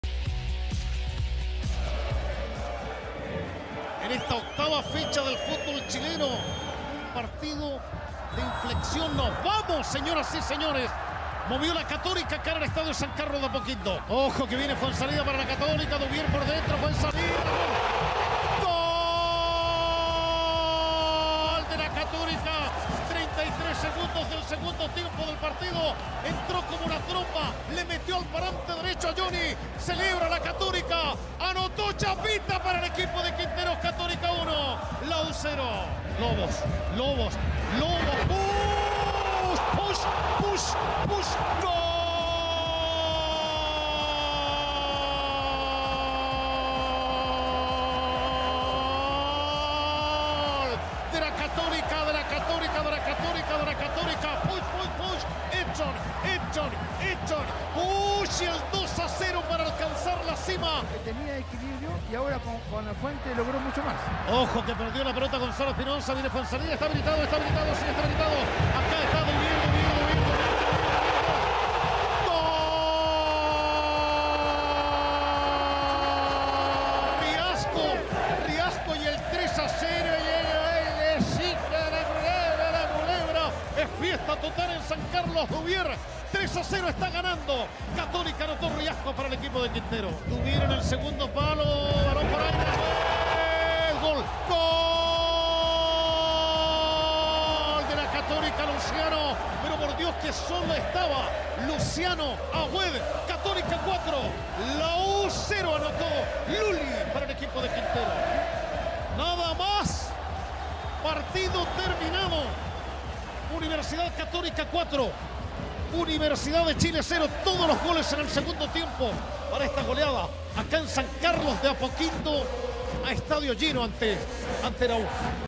2019: U. Católica goleó a placer a la U en San Carlos
El 14 de abril de 2019 la Universidad Católica hizo lo que quiso en casa, San Carlos Apoquindo, ante su clásico rival Universidad de Chile y en CDF Noticias te invitamos a recordarlo.

José Pedro Fuenzalida, Edson Puch, Duvier Riascos y Luciano Aued sellaron la goleada del conjunto dirigido en ese entonces por Gustavo Quinteros.